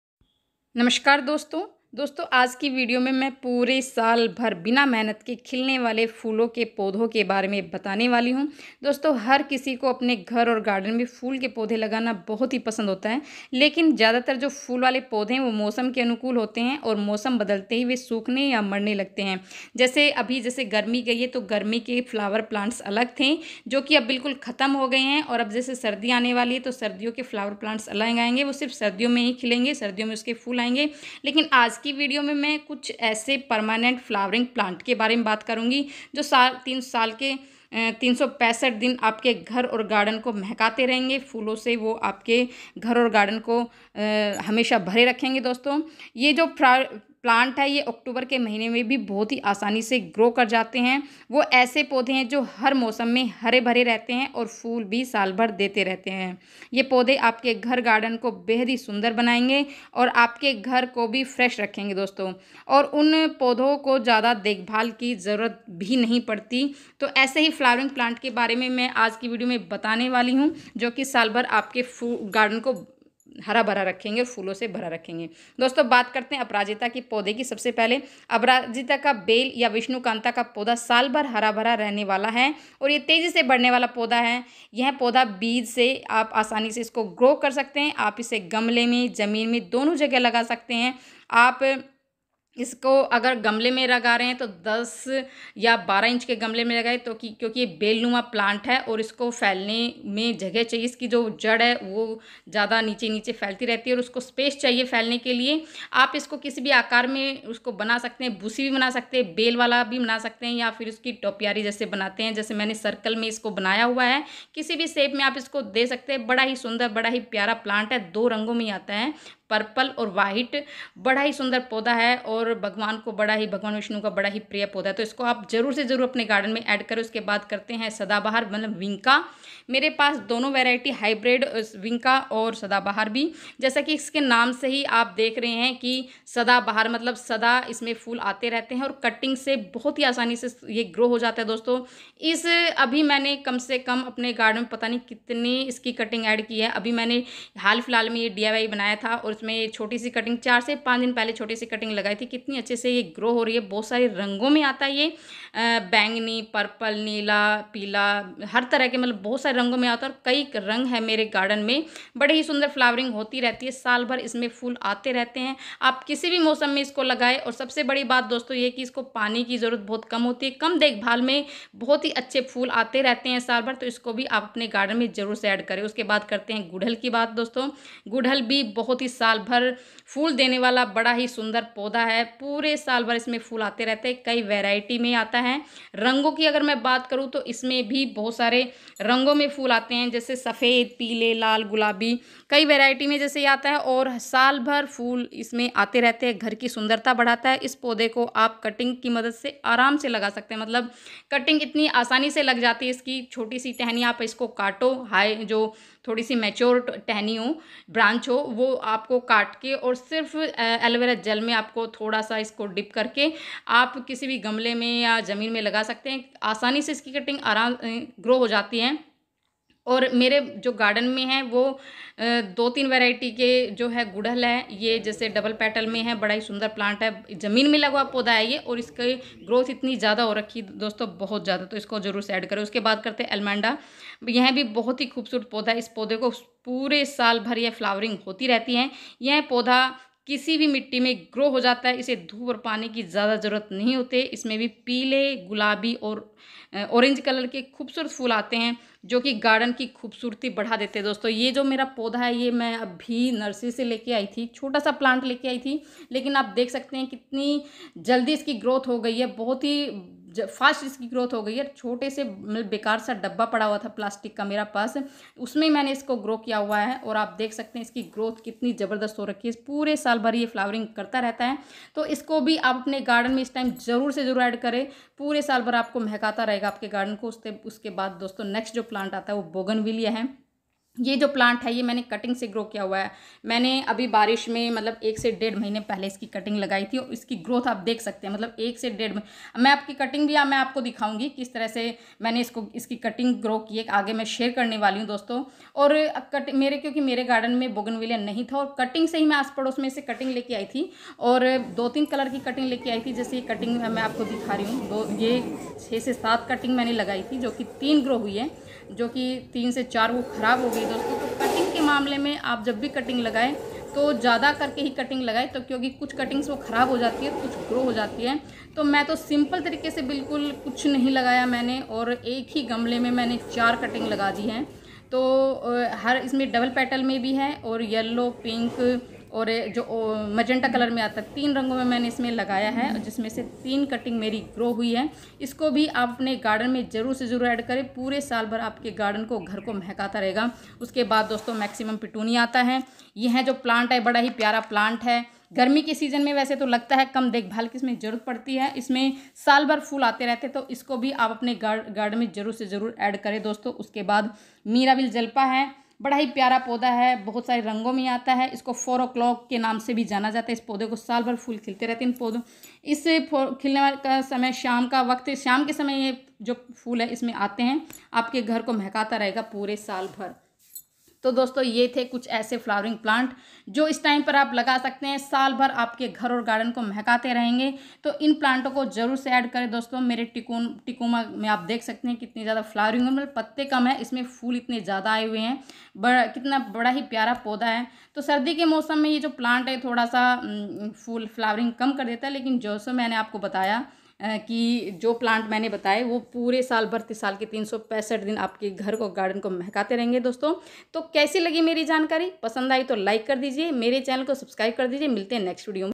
Gardening tips: साल भर बगीचे में खूब खिलेंगे फूल गमले में लगा ले 7 सस्ते टिकाऊ पौधे Low maintenance
Gardening tips: साल भर बगीचे में खूब खिलेंगे फूल गमले में लगा ले 7 सस्ते टिकाऊ पौधे Low maintenance plants 👍
gardening with Santosh 🌴🌳